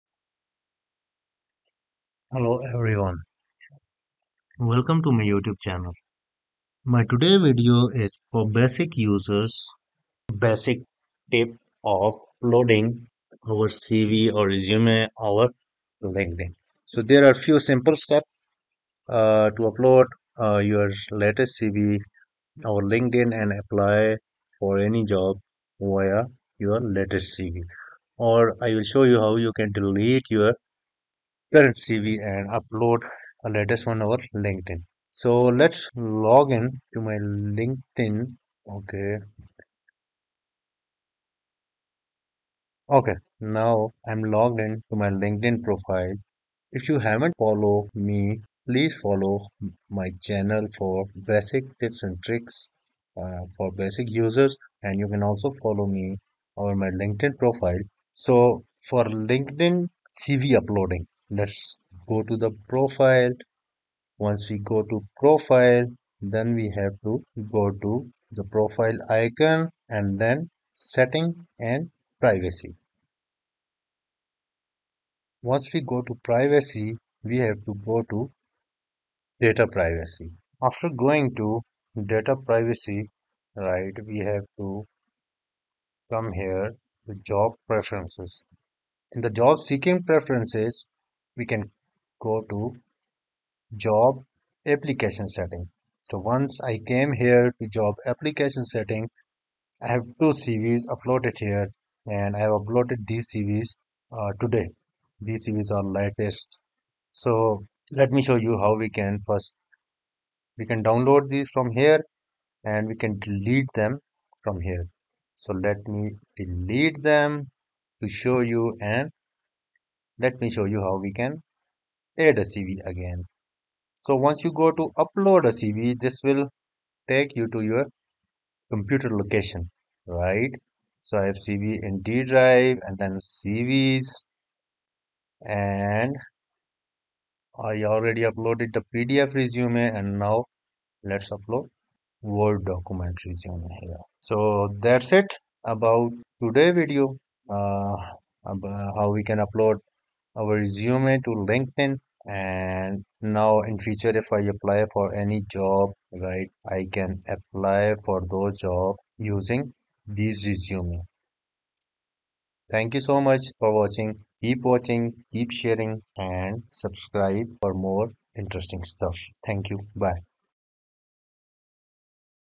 How to upload or delete resume on LinkedIn | Step-by-Step Guide
In this tutorial video, we will walk you through the process of uploading a new resume on LinkedIn for both basic and advanced computer users. Whether you are looking to update your profile for job applications or simply want to manage your resume on the platform, we've got you covered.

We will show you step-by-step instructions on how to upload the latest resume to apply for a job, as well as how to delete an existing resume from your LinkedIn profile. By following our easy-to-understand guide, you will be able to make the necessary changes to your LinkedIn profile with confidence.

Don't miss out on this essential tutorial for maximizing your job search efforts on LinkedIn. Watch now and take control of your professional online presence!